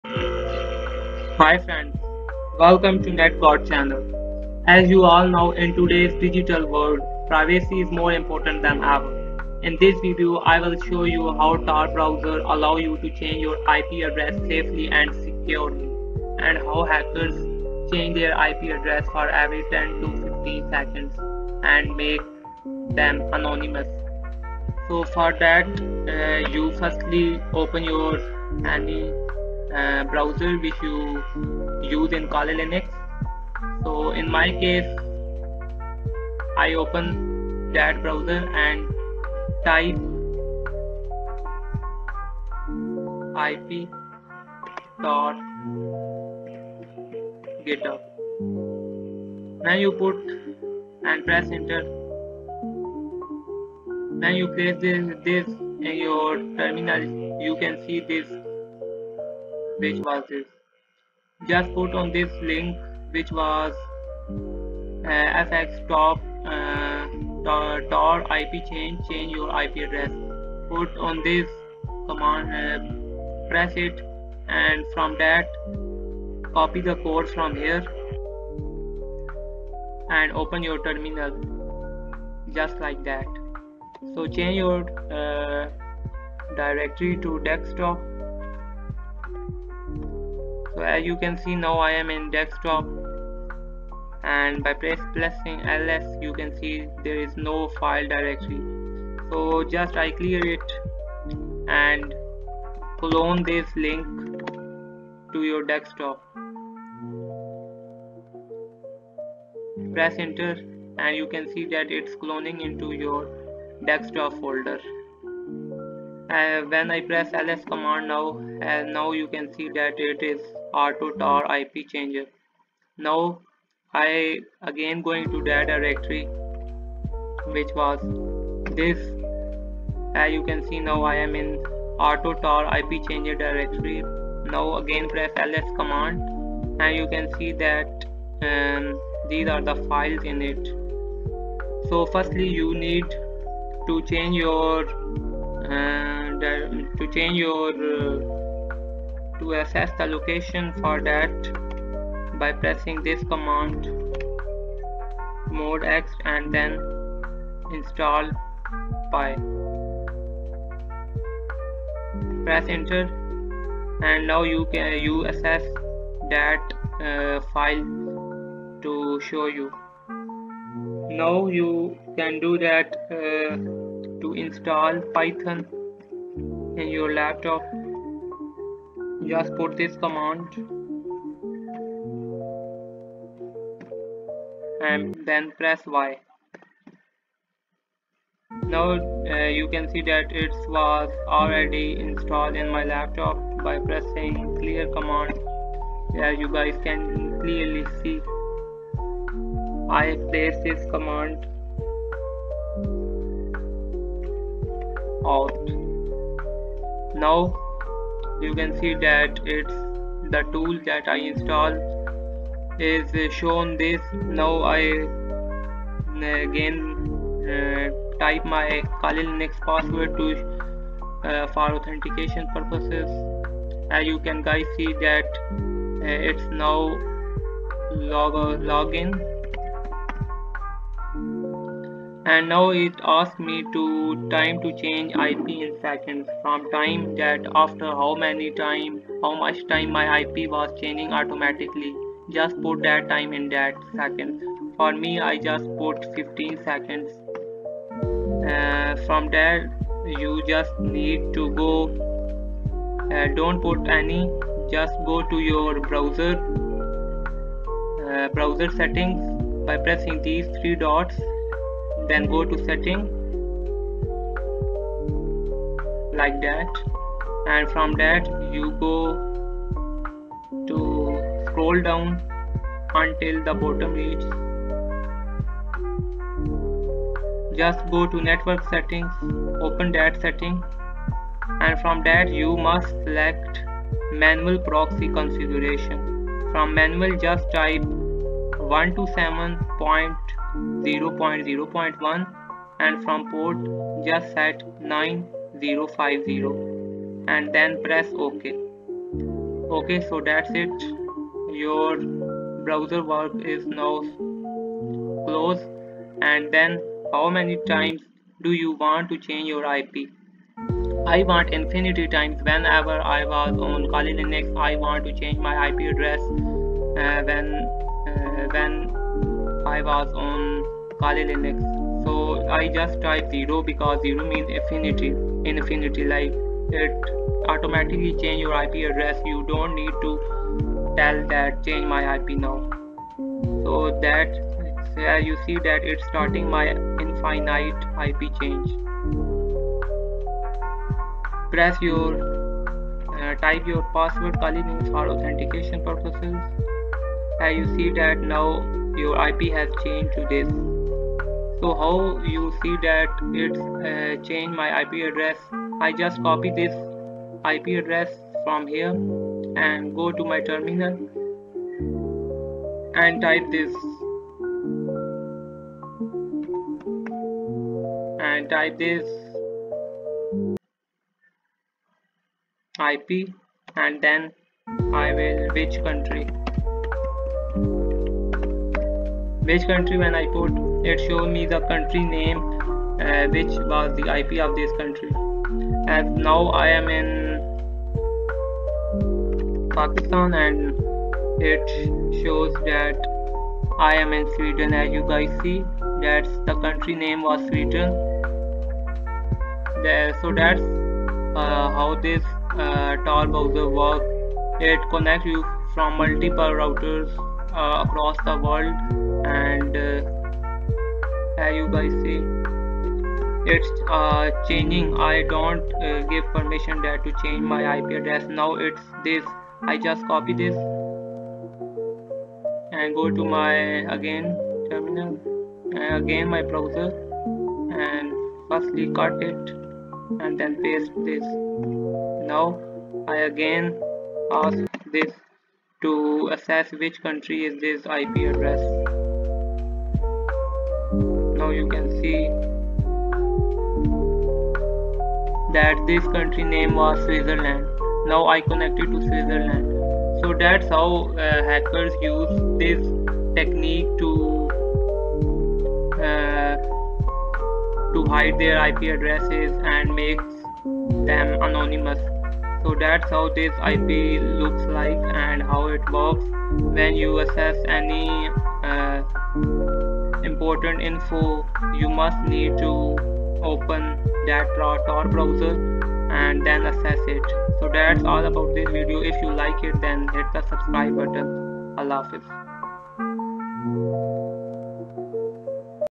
How to Change Your IP Address Every 5 Seconds Automatically 2025 | Kali Linux
🔒 Stop being tracked online! This FREE Python script automatically changes your Tor IP address every 5 ethicall seconds for maximum anonymity.

In this step-by-step tutorial, I'll show you how to set up and run an automatic IP rotator using Tor on your system. Enhance your privacy and security with this powerful tool.

This method is perfect for penetration testers, security researchers, and anyone serious about their online privacy.